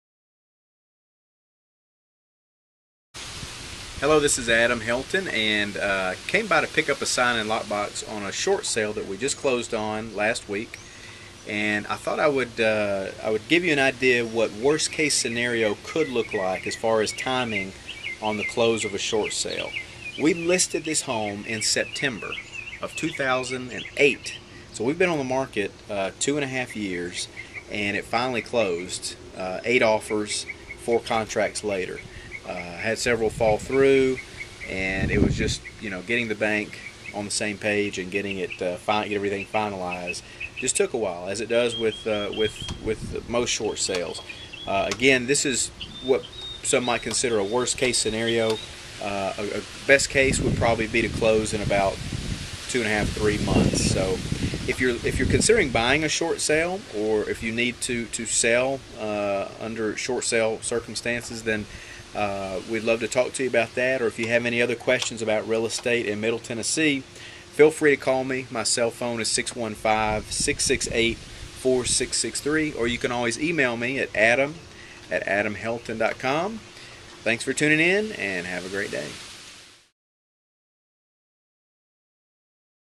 Short Sales: Worst Case Scenario?
Adam talks about what could be a worst case scenario regarding the time frame to close a short sale.  If you have questions about Real Estate in Middle Tennessee, please email . You can also search for homes in Middle Tennessee